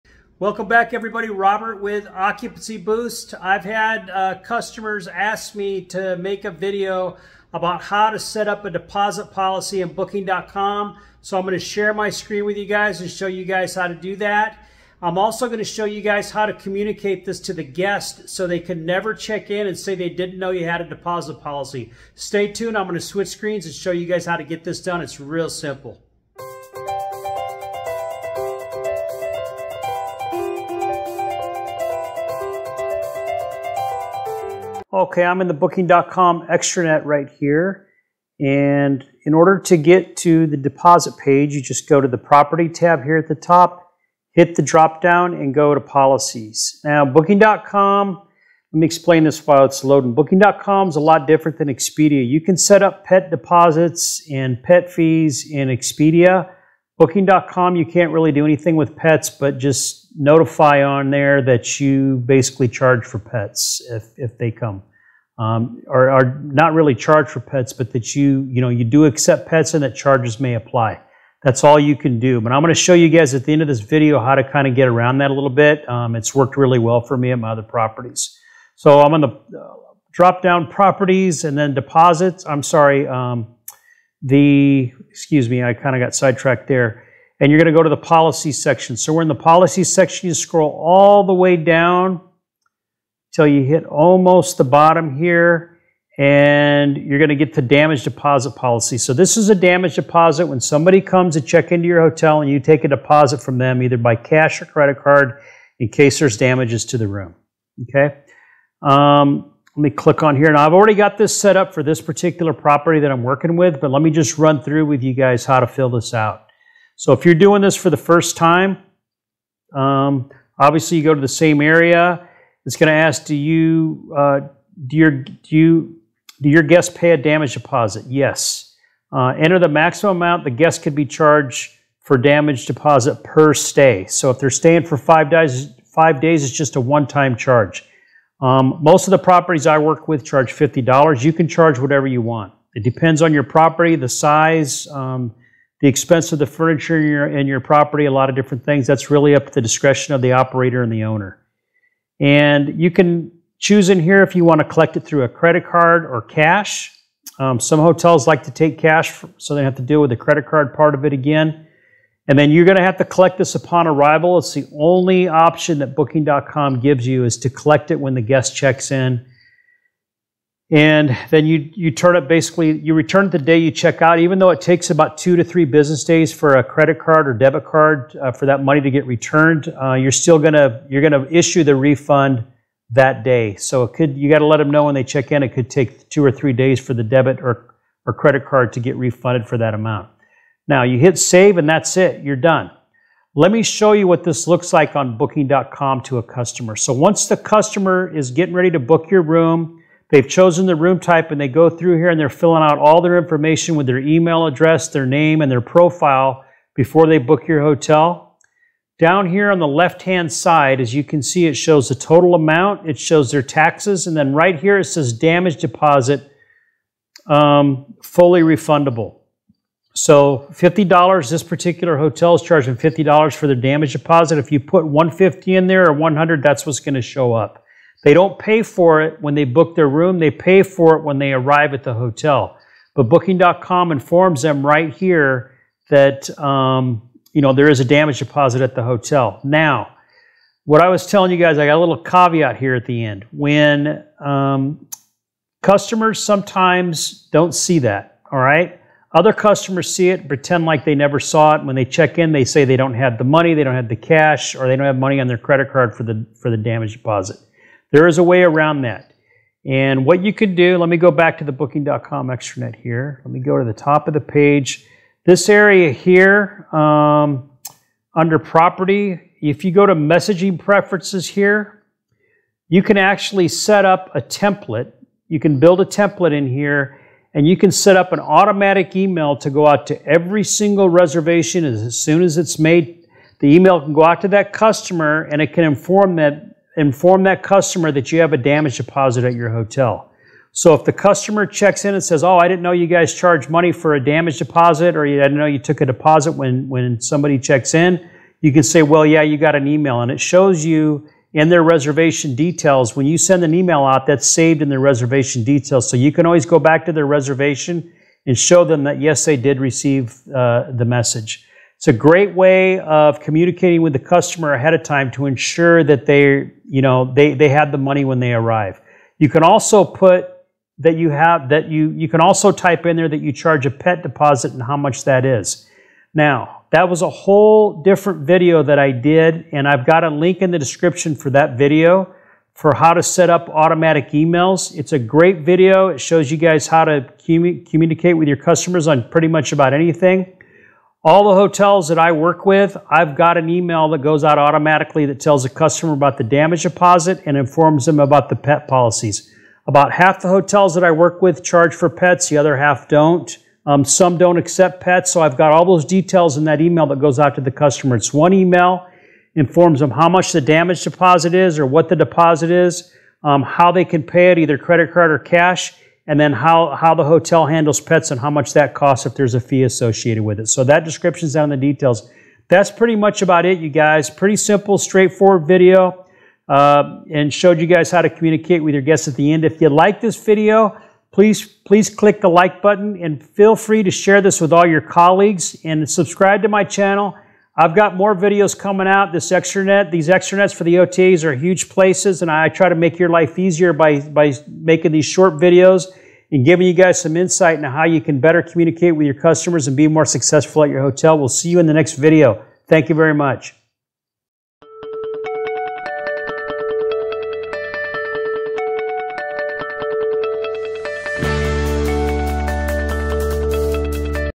How to set up a Deposit Policy Booking com
Does your hotel collect a damage deposit or other types of deposits at check in?  In this video I show you step by step how to set up a check in Deposit Policy in the Booking.com Extranet.

Sometimes the hardest part about managing a hotel is knowing where to find answers to simple online questions and how to solve problems. The Booking.com Extranet houses a massive amount of data and sometimes finding what you need is tough to find.

(Video Referenced in this Video) Set up Automatic Pre Arrival Emails to all guests to remind them about your required deposit

Watch a great follow up to this video here on How To Set up a Deposit policy on Expedia